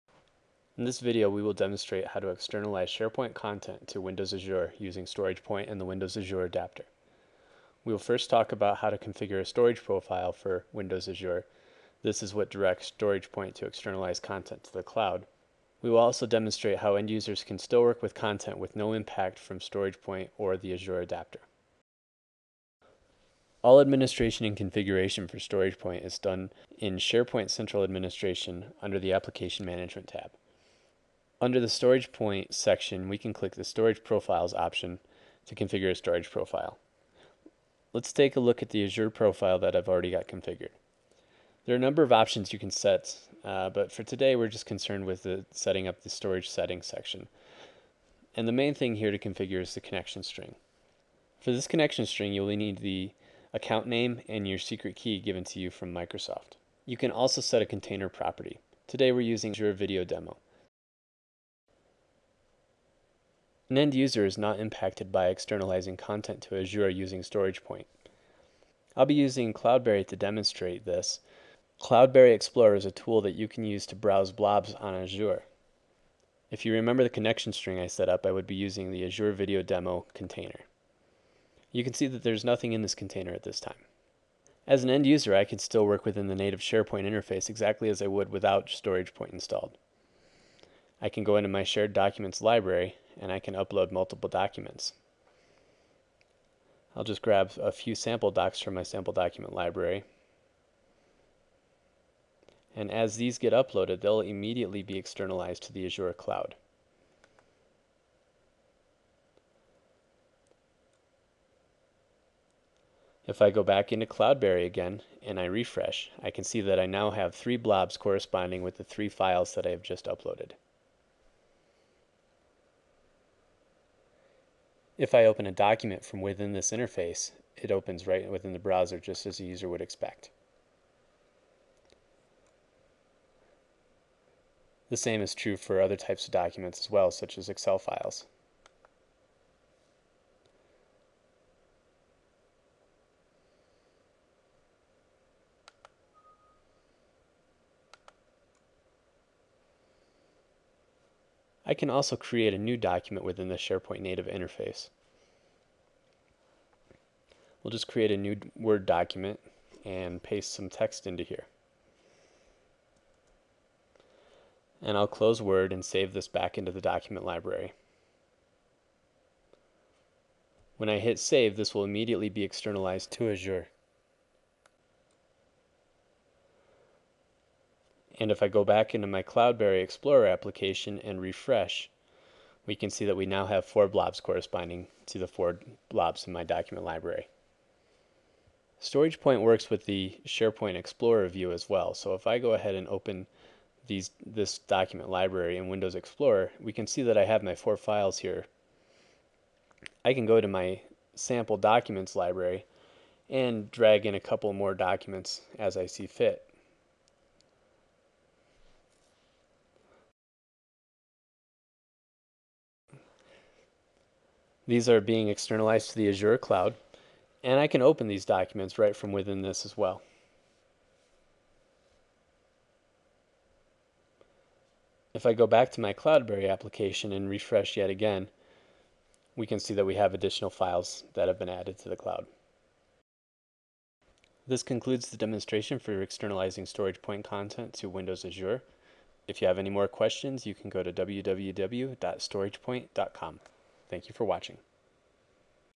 StoragePoint Windows Azure BLOB Storage Adapter
A demonstration of StoragePoint externalizing SharePoint content to Windows Azure BLOB Storage